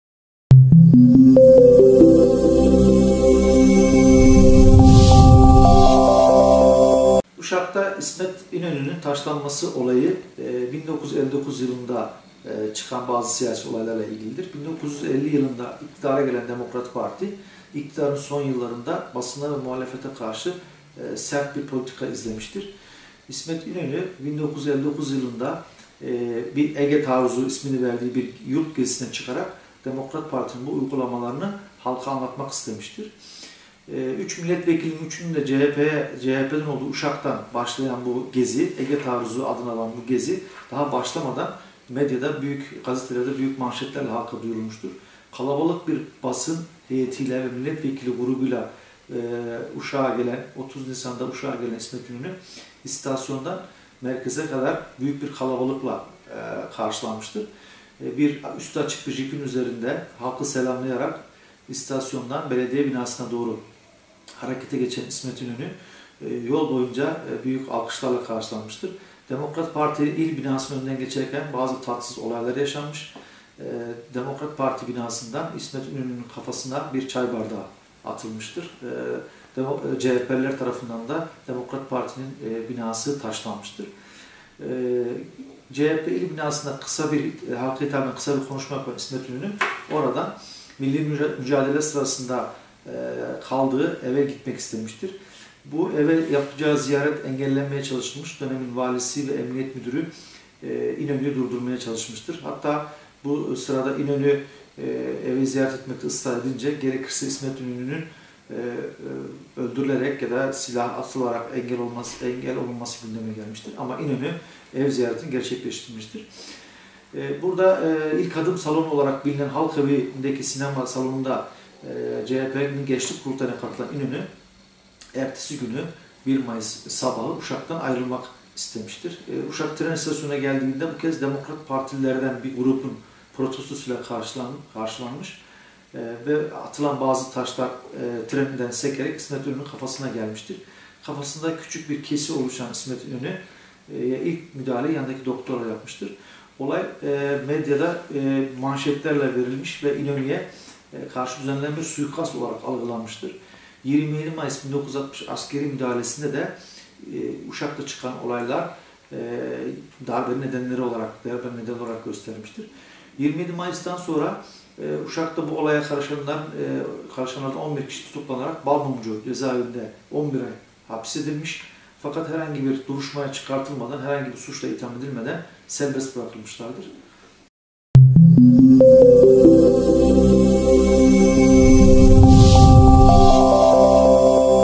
İsmet İnönü'nün Uşak ziyareti sırasında taşlanması olayı
Doç. Dr. Mehmet Karayaman, Uşak Üniversitesi Tarih Bölümü